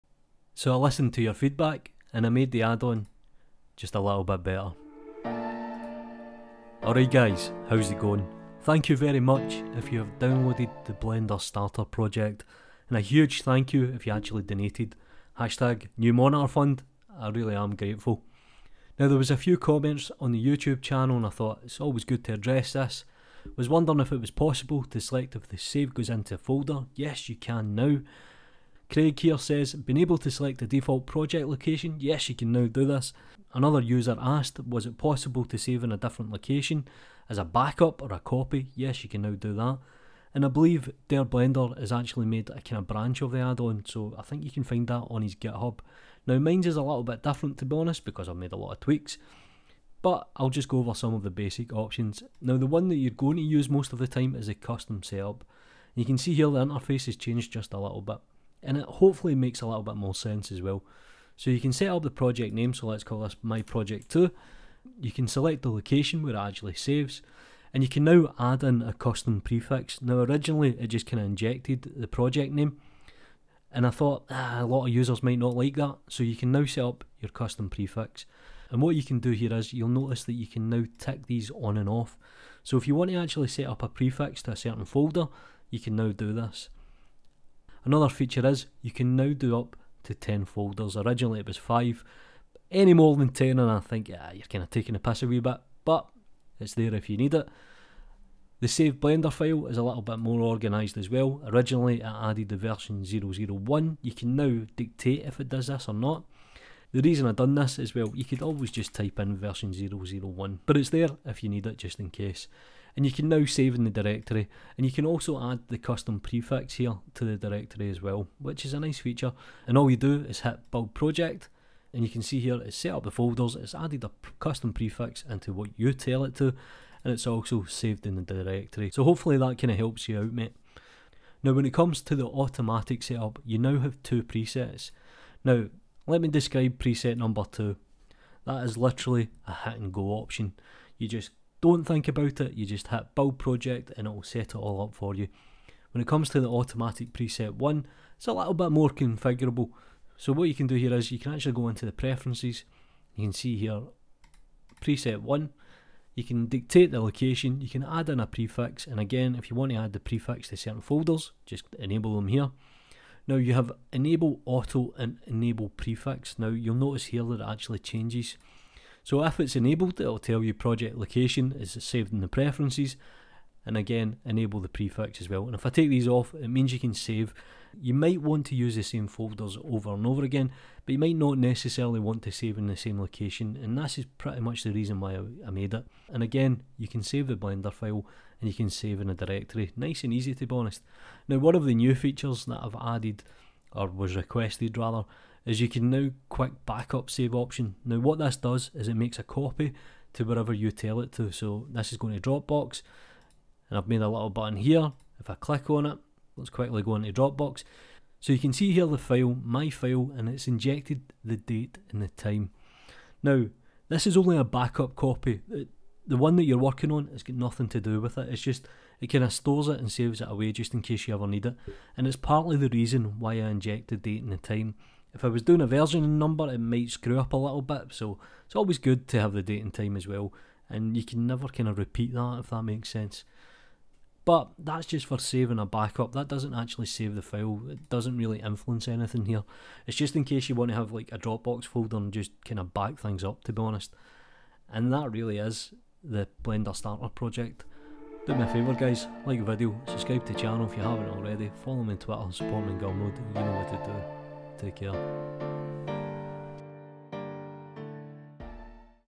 ⚠️Blender Project Starter Addon - Update
A tool for creating folders and starting a blender project.